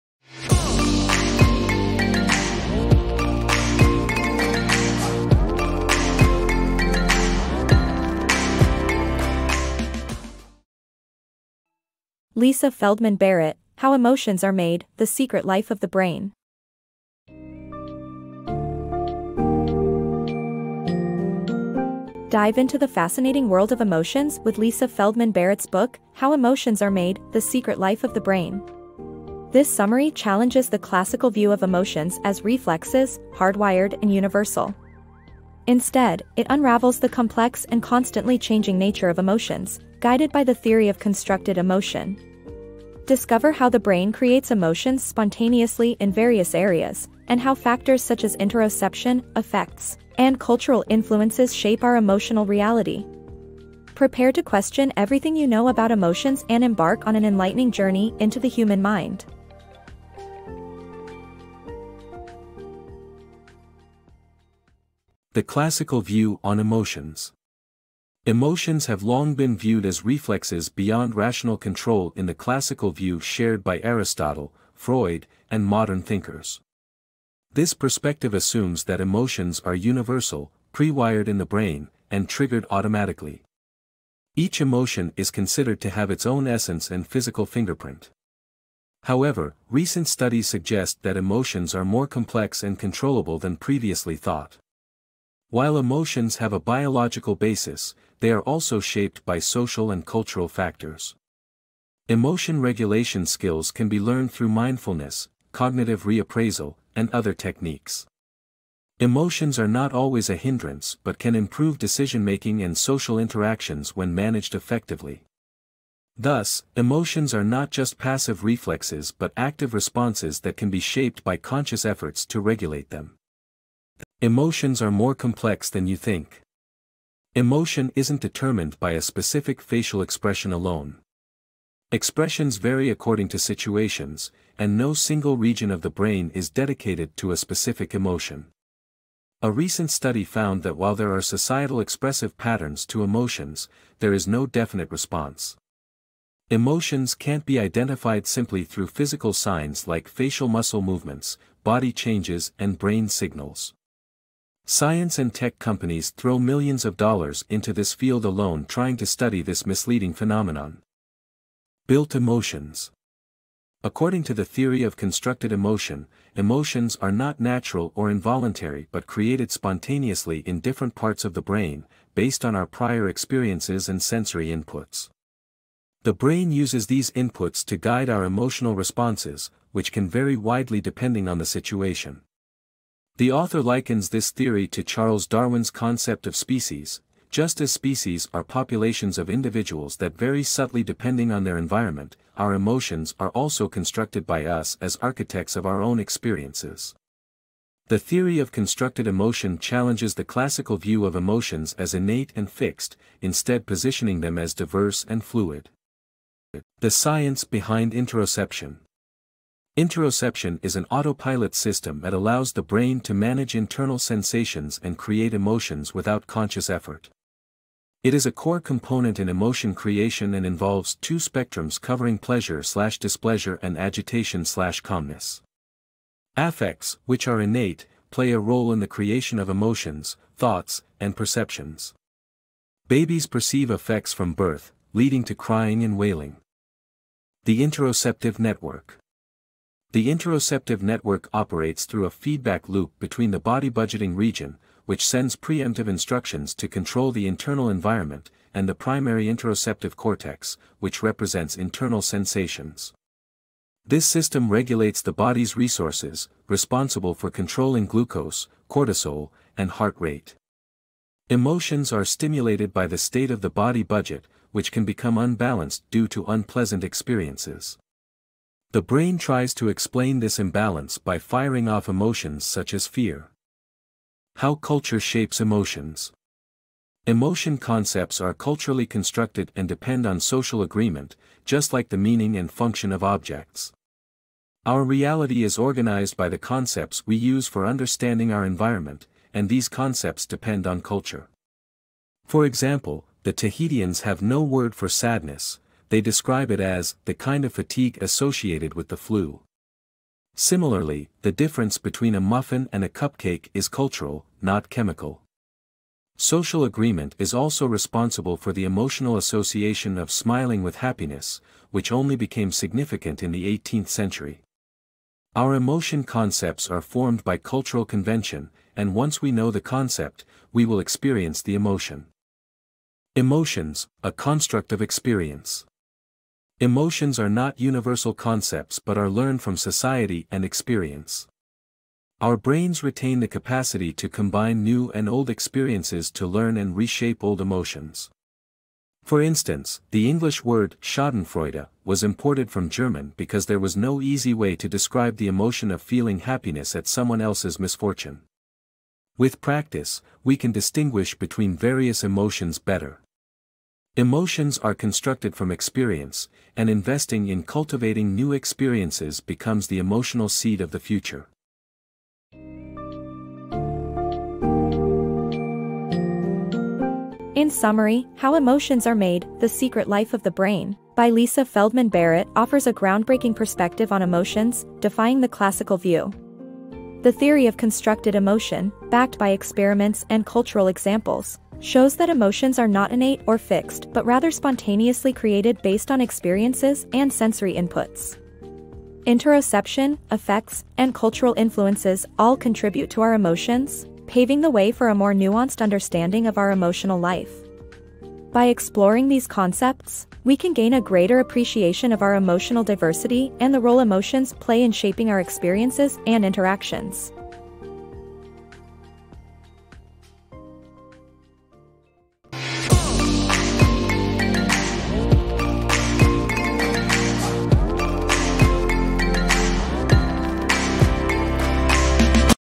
How Emotions Are Made by Lisa Feldman Barrett: 7 Minute Summary
*BOOK SUMMARY*

TITLE - How Emotions Are Made: The Secret Life of the Brain

AUTHOR - Lisa Feldman Barrett

DESCRIPTION:
📖 Discover a groundbreaking new theory on the science of emotions with How Emotions Are Made. Psychologist and neuroscientist Lisa Feldman Barrett reveals how emotions are not hardwired in the brain but instead constructed in the moment and shaped by our lifetime of learning. Explore the implications of this paradigm-shift for psychology, healthcare, law enforcement, and more.

TIMESTAMPS:
⌚ 00:00 Introduction
⌚ 01:06 The Classical View on Emotions
⌚ 02:04 Emotions are More Complex Than You Think
⌚ 02:42 Built Emotions
⌚ 03:27 The Science Behind Interoception
⌚ 03:58 The Interoceptive Network
⌚ 04:34 How Culture Shapes Emotions
⌚ 05:23 Emotions: A Construct of Experience
⌚ 06:02 Final Recap